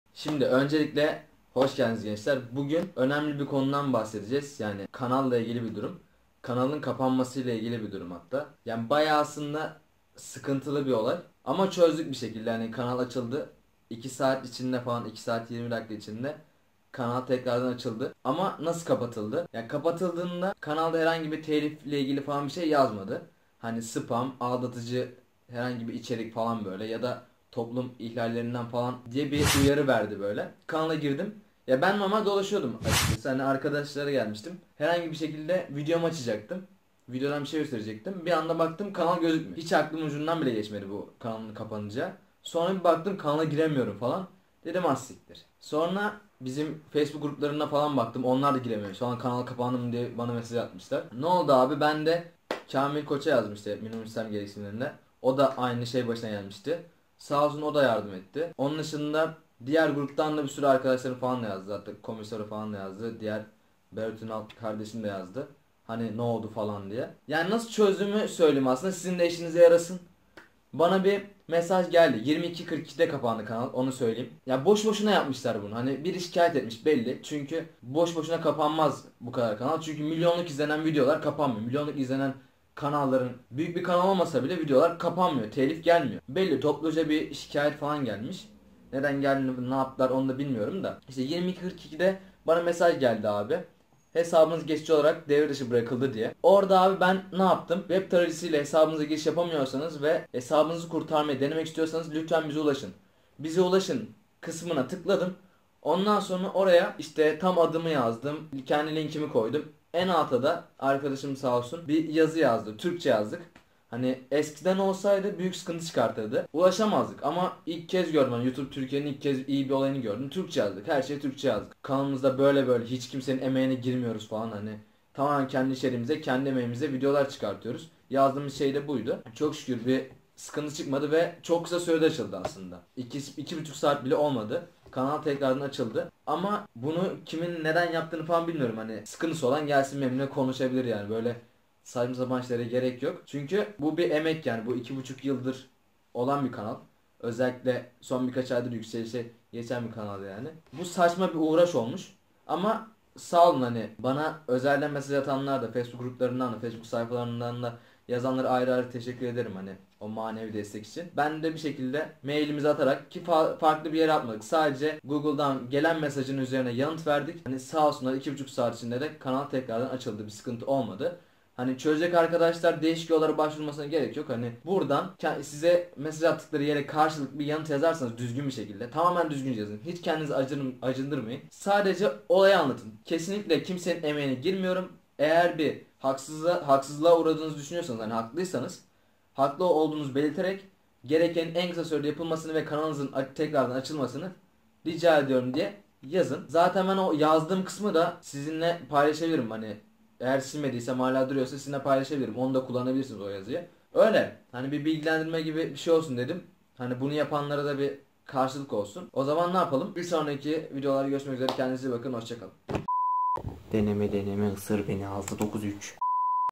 KANALIM NEDEN KAPANDI - NASIL AÇILDI ?
Dün kanalım nedensiz ve açıklamasız şekilde aniden kapandı. Kanalı nasıl açtım, kanal neden kapandı, anlatıyorum.

İtiraz Yazısı : Öncelikle iyi çalışmalar. Ben kendi hakkımı, emeğimi savunduğum gibi başkalarının emeğine hakkına da saygı duyarım. Bu kanala tamamen kendi emeğimle saatlerce uğraşarak videolar hazırlayıp abonelerimle paylaşıyorum. Şuan karşılaştığım durum beni çok şaşırttı. Telif hakkı nedeniyle hesabımın askıya alınması durumunda bir hata olduğunu düşünüyorum. Bu telif hakkı konusuna çözüm getirilmesini ve en kısa sürede hesabımın açılmasını saygılarımla rica ediyorum.

youtube kanalı nasıl açılır
youtube kanalım kapandı